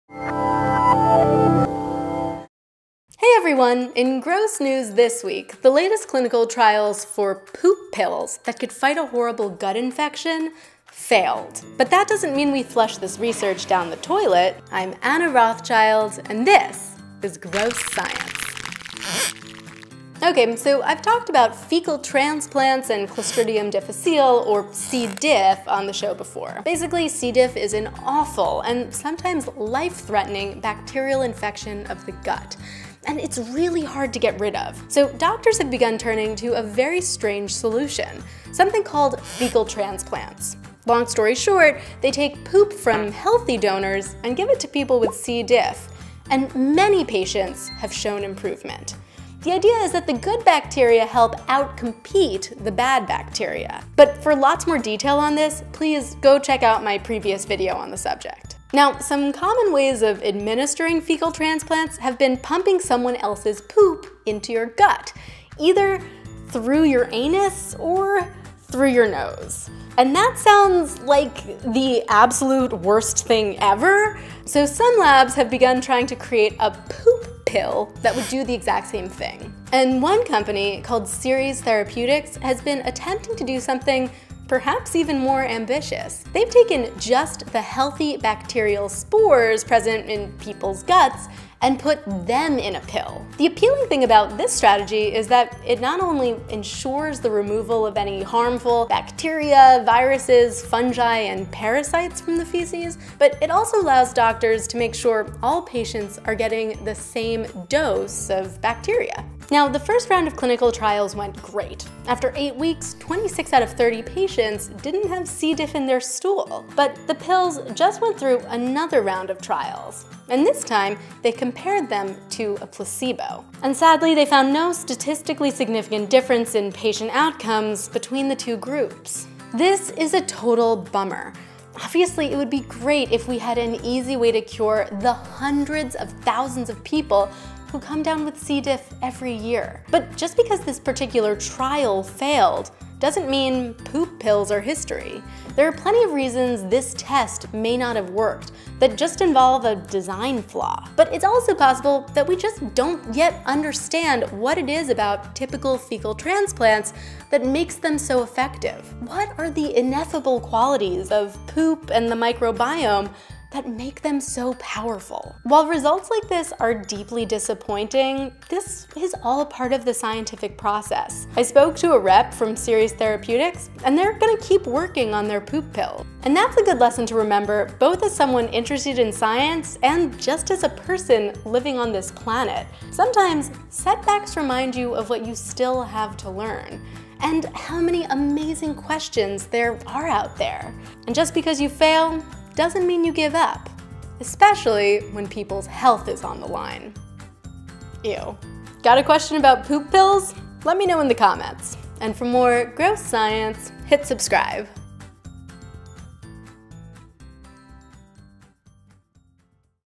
How (and Why) to Make a Pill Out of Poop
The latest clinical trials for poop pills that can fight a gut infection failed. But that's no reason to flush this research down the toilet.
↓Want more info?↓

The Journal of Infectious Diseases: A Novel Microbiome Therapeutic Increases Gut Microbial Diversity and Prevents Recurrent Clostridium difficile Infection

The New England Journal of Medicine: Burden of Clostridium difficile Infection in the United States

Host, Writer, Animator
Anna Rothschild

Camera, Editor
Sara Tewksbury

Footage and Images

Clostridium difficile
CDC

IMG_1760 (dog behind)
Flickr/50-phi

Super nose
Flickr/montillon.a

Energinzing?
Flickr/Patrik Nygren

SalmonellaNIAID
Wikimedia Commons/NIAID

Norovirus
CDC/Charles D. Humphrey

Candida albicans PHIL 3192 lores
Wikimedia Commons/CDC/Dr. William Kaplan

Giardia intestinalis (259 17)
Wikimedia Commons/Doc. RNDr. Josef Reischig, CSc.

HK Chai Wan IVE 藥劑實驗室 Pharmaceutical Science Laboratory 糖衣 Sugar coated Room 516
Wikimedia Commons/Chlaborewan

FMTvials
Wikimedia Commons/Cjc2nd

SFX

Cockroaches
Freesound/StateAardvark­

Squeak Pack/squeak_10
Freesound/Corsica_S

Short fart 01
Freesound/dsisstudios

Wink
Freesound/bennychico11

Marker Writing on Paper
Freesound/ftpalad

Produced by WGBH for PBS Digital Studios